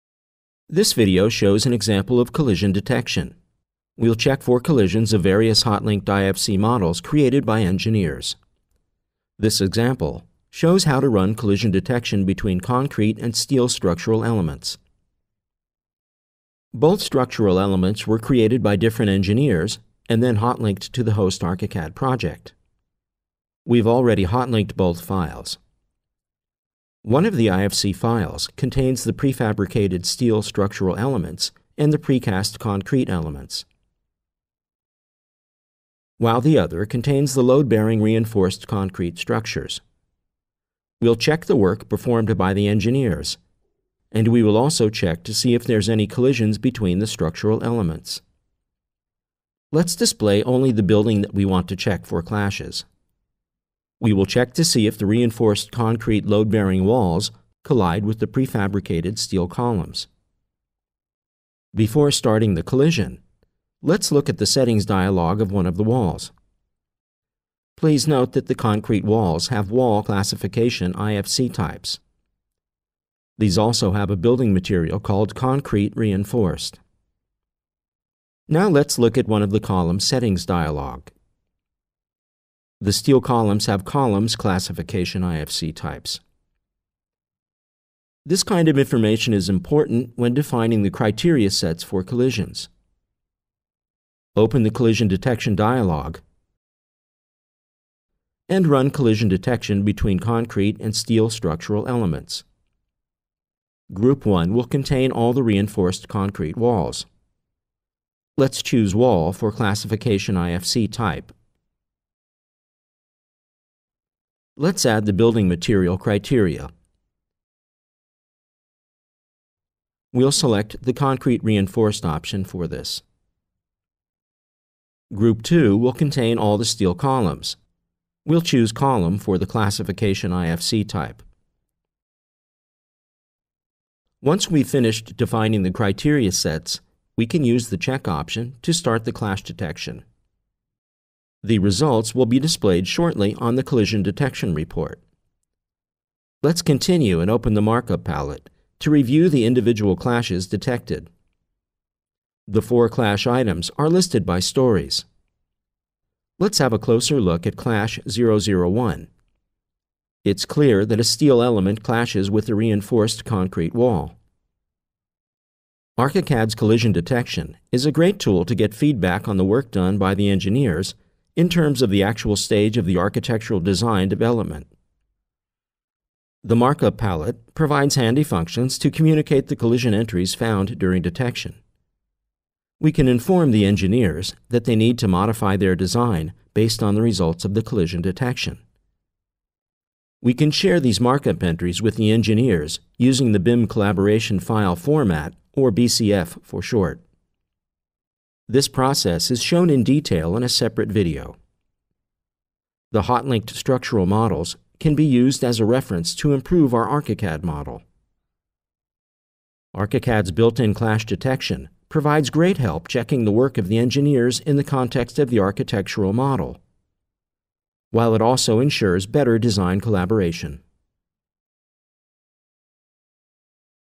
Collision Detection in ARCHICAD 21 - Between Concrete and Steel Structural Elements
The hotlinked structural models can be used as a reference to improve our ARCHICAD model. ARCHICAD's built-in clash detection provides great help checking the work of the engineers in the context of the architectural model while it also ensures better design collaboration. This video shows an example of Collision Detection. We’ll check for collisions of various hotlinked IFC models, created by engineers. This example shows how to run collision detection between concrete and steel structural elements.